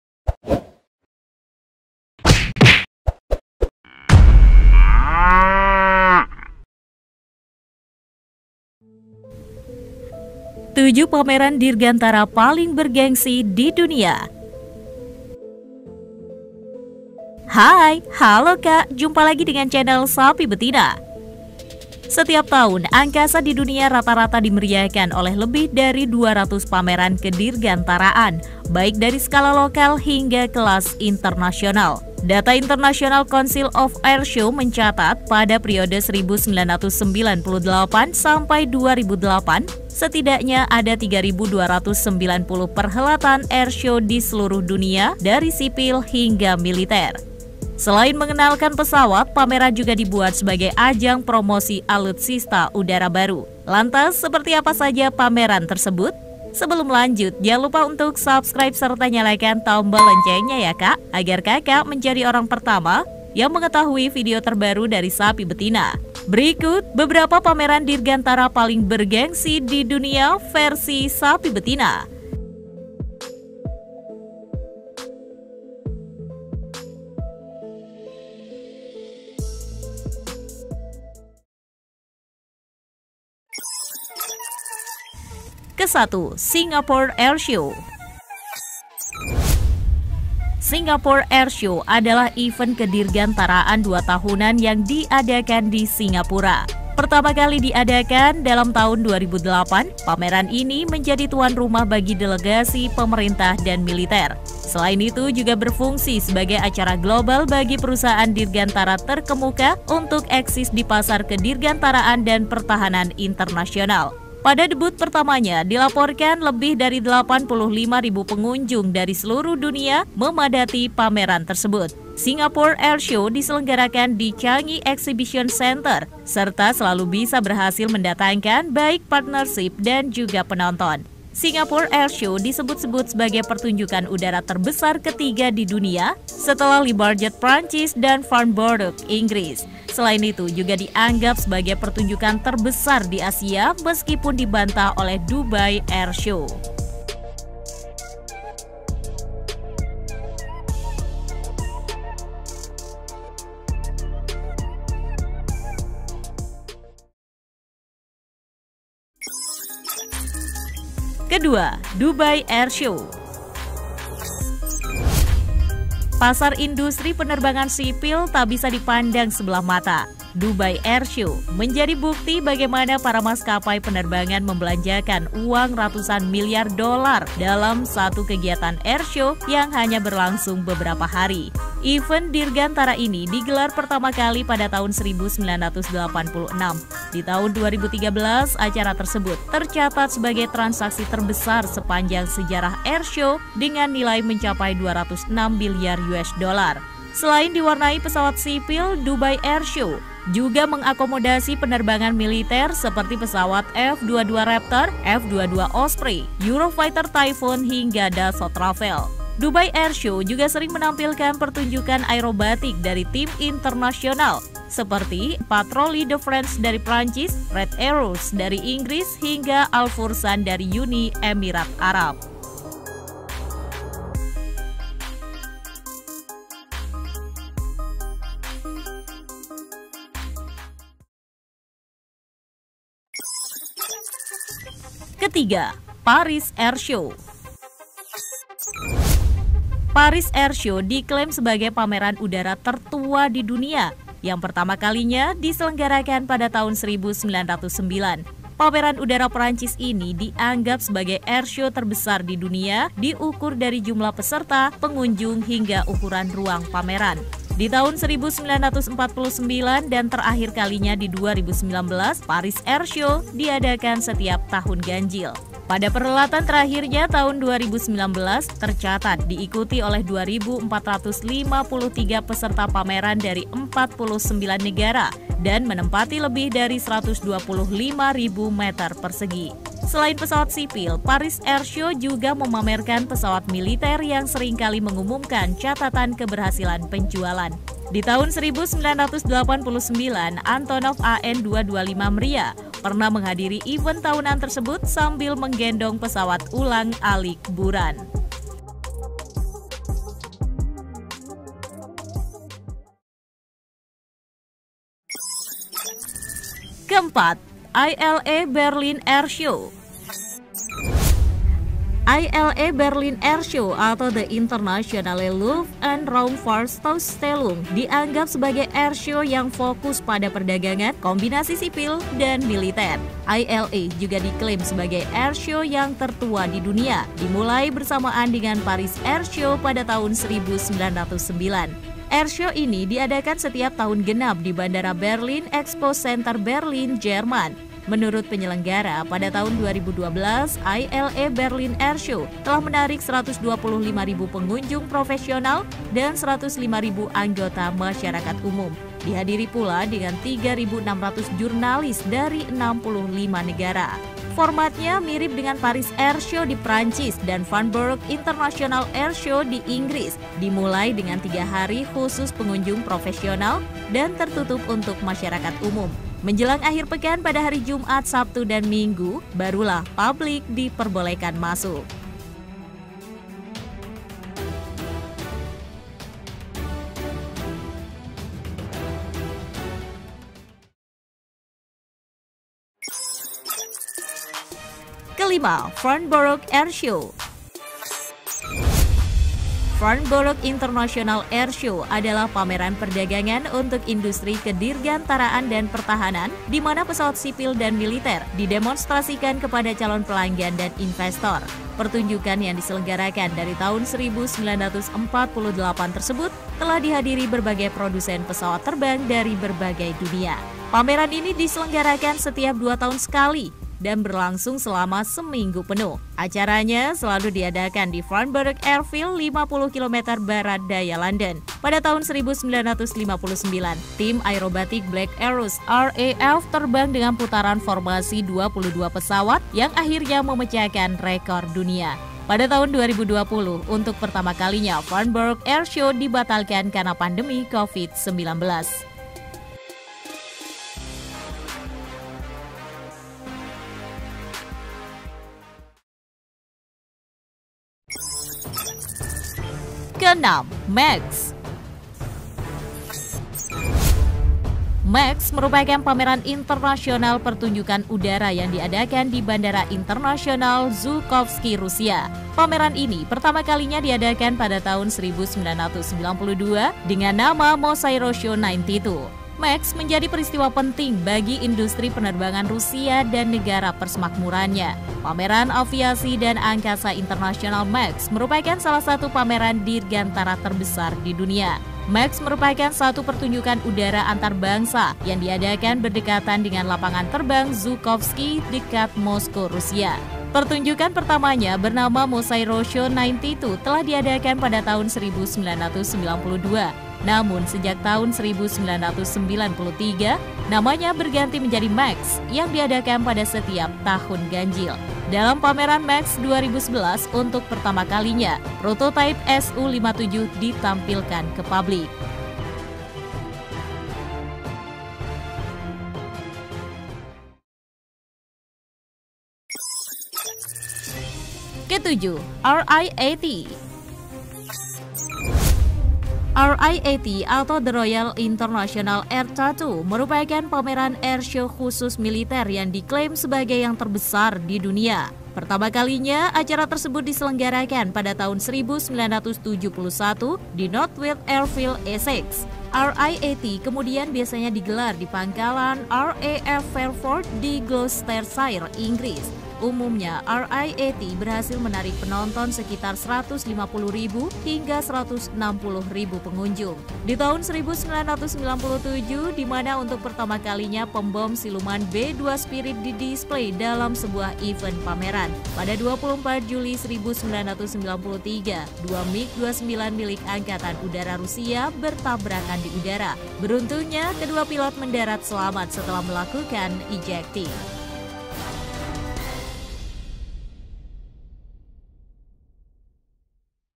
PAMERAN SEKALIGUS JUALAN! Deretan Pameran Dirgantara Paling Bergengsi di Dunia
Setiap tahun, angkasa di dunia rata-rata dimeriahkan oleh lebih dari 200 pameran kedirgantaraan, baik dari skala lokal hingga kelas internasional.
Data International Council of Air Shows mencatat pada periode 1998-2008 setidaknya ada 3.290 perhelatan airshow di seluruh dunia dari sipil hingga militer.
Selain mengenalkan pesawat, pameran juga dibuat sebagai ajang promosi alutsista udara baru.
Seperti apa saja pameran tersebut?